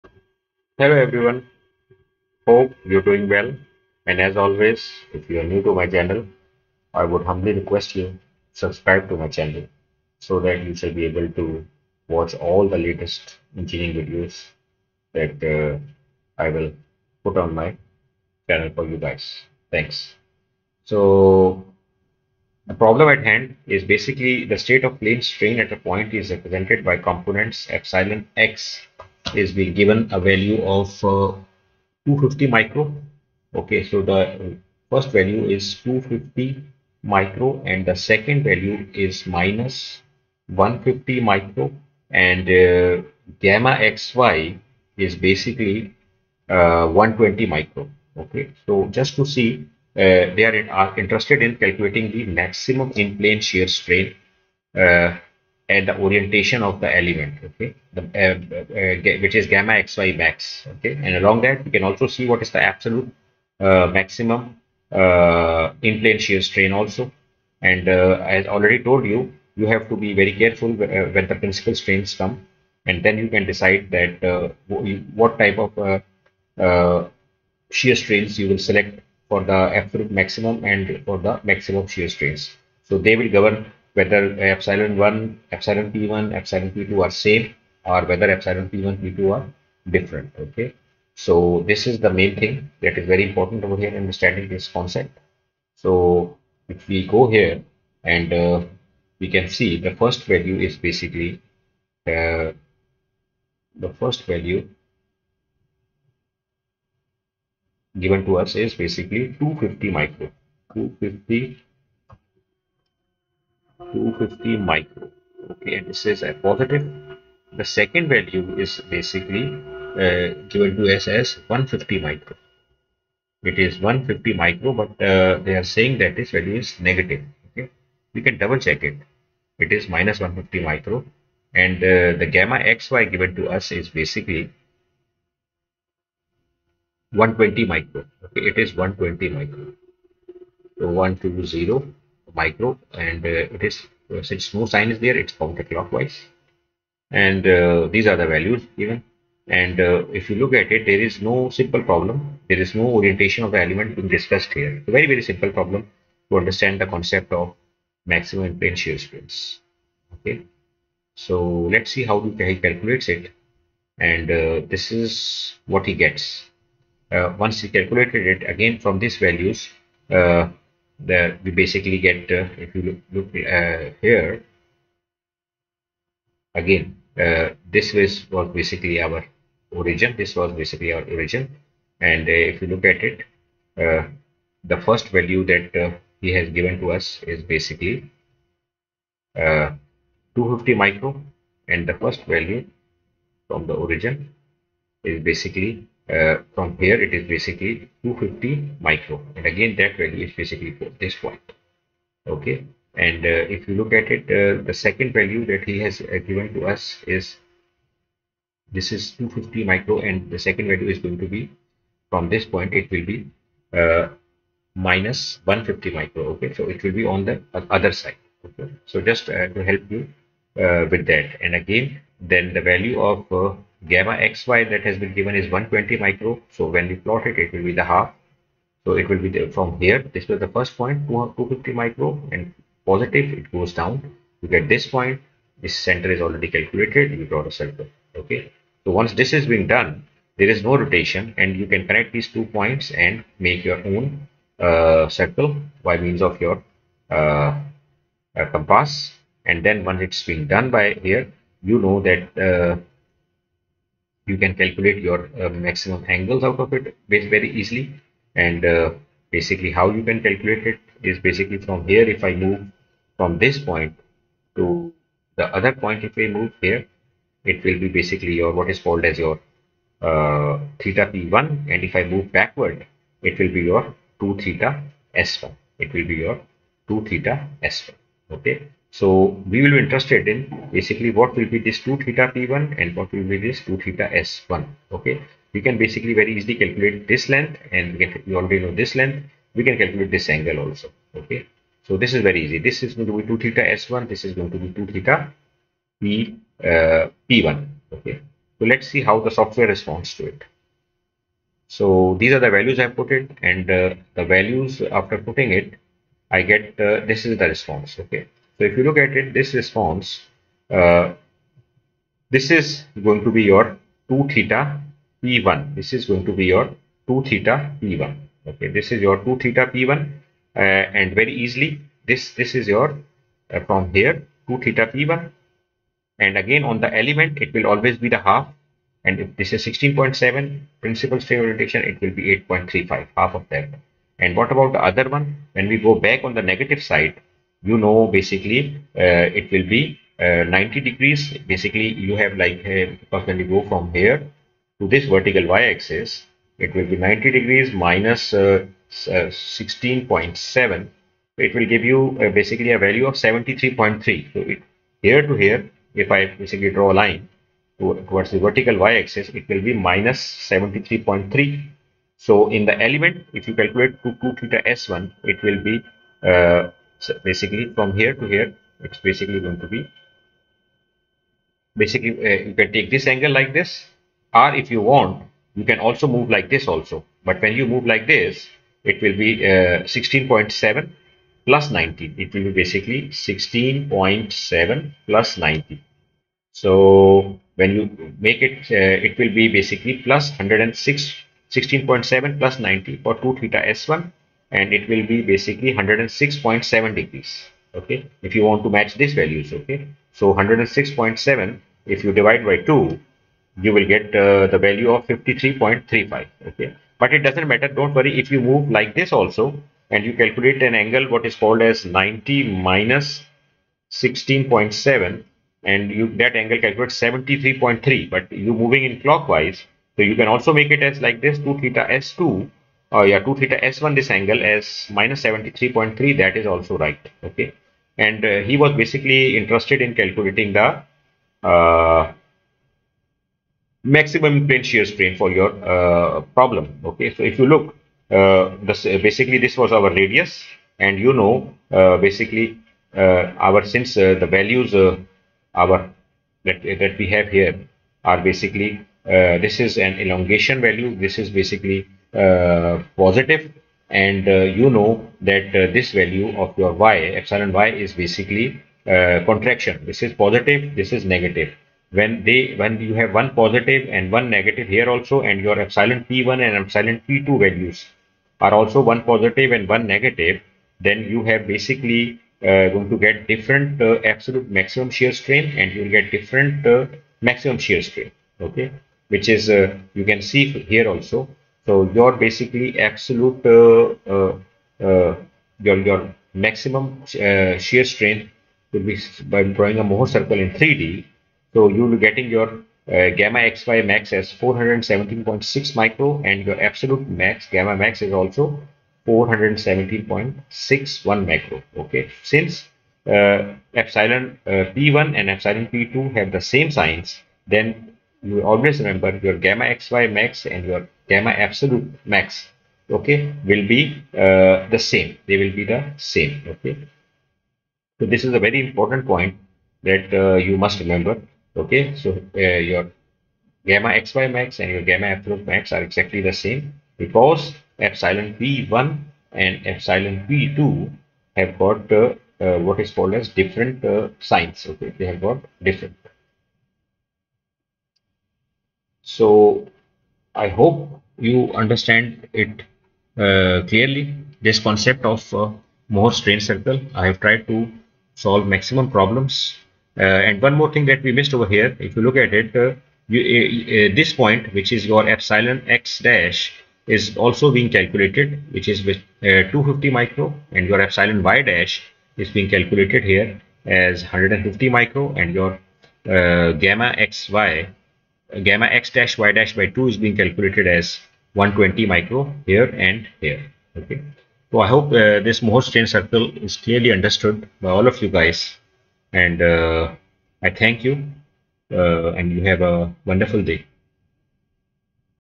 Mohr strain circle Absolute Maximum and maximum shear stress numerical on MDS
In this video, we will validate a book solved numerical of MOHRS stain circle, on the software MDS, in which we study about the difference in absolute maximum and maximum shear stresses and how they relate to each other, in comparison to the maximum shear strains